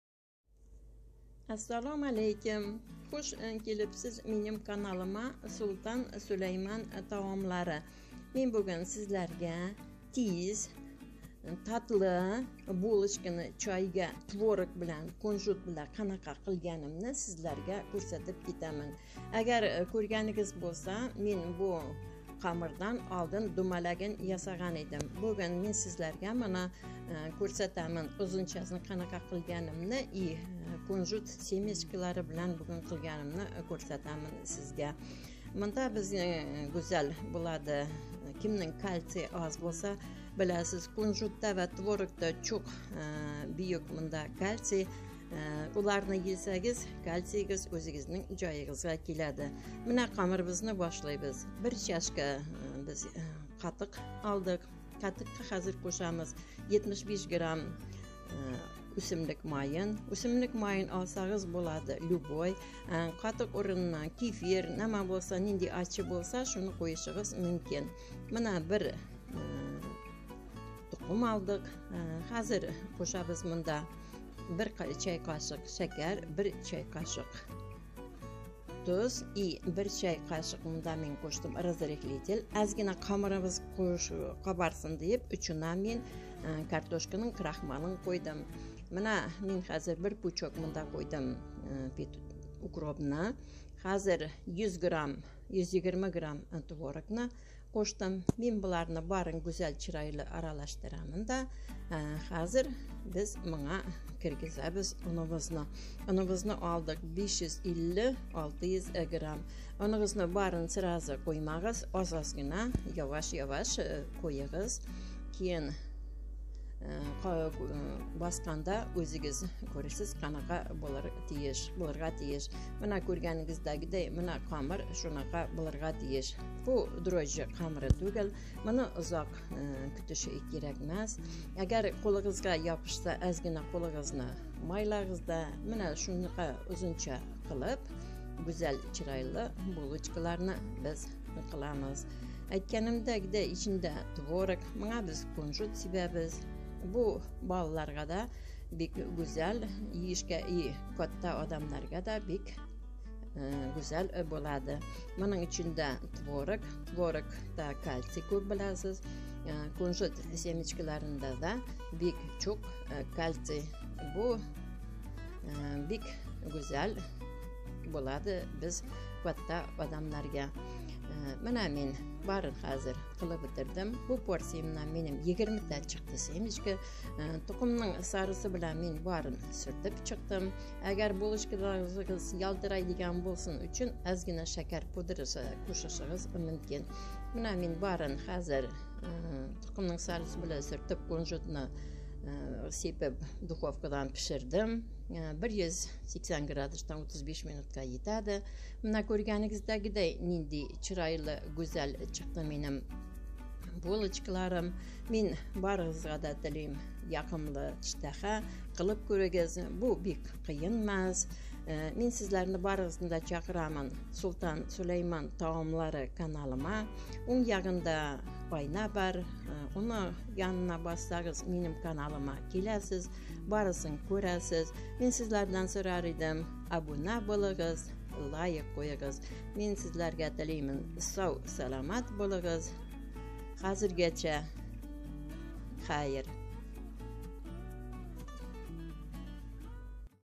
Tez. CHOY UCHUN MAZALI BULKA.PISHLOQ VA SUSAM BILAN.-- QANDAY QILISH KERAK?
BUGUN MEN ODDIY , ODDIY , MAZALI PISHIRAMAN . MAVJUD PODUKTOVDAN MAVJUD BULKA.
MEN BUNDAN OLDIN DUMALOQ PISHIRDIM, BUGUNGI KUNDA ULAR SUSAM BILAN CHO'ZILADI.
XAMIR UCHUN SIZGA KERAK BO'LADI:
100 gr. NORDON SUT.
100 gr. O'SIMLIK YOG'I.
2 YAITSO. 1 SARIG'I 100-125 GR. Pishloq.
1 BIR SHINGIL. NOZIK KESILGAN.
1 choy qoshiqda tuz, shakar. SODA VA BIR OZ KARTOSHKA KRAXMAL.
ELAKDAN ELENMIŞ UN 500-600 GRAMM.
SOS UCHUN BUG'DOYNI BO'YASH UCHUN.
1 TUXUM SARIG'I.
Qavat. Choy qoshiq shakar kukuni,
1 choy qoshiq o'simlik yog'i.
XAMIRNI TAYYORLAGANDAN SO'NG, BIZ BUG'DOYNI YOPAMIZ.,
USHBU QISMLARDAN 20 TA BUG'DOY OLINADI.
TAYYOR BUG'DOY SARIG'I BILAN TARQALADI. 180-30 DAQIQA DAVOMIDA OLDINDAN QIZDIRILGAN 35 DARAJA O'CHOQQA SOLINGAN SESAME TUSHIRING .
ÇÖREKLER JUDA CHIROYLI . Oltin . ÇITIR VA JUDA MAZALI CHIQDI, ÇÖREKLER TAYYOR.
BIZNING STOLIMIZNI SERVERLAYMIZ .
HAMMANI STOLGA TAKLIF QILAMAN ,
HAR BIR INSON YOQIMLI TUYADI !!1
MEN YOUTUBE KANALIMNI XALQLARNING MAZALI TAOMLARINI TAKLIF QILAMAN. E'TIBORINGIZ UCHUN TASHAKKUR.
HOZIRGACHA. HAMMA UCHUN MUVAFFAQIYAT.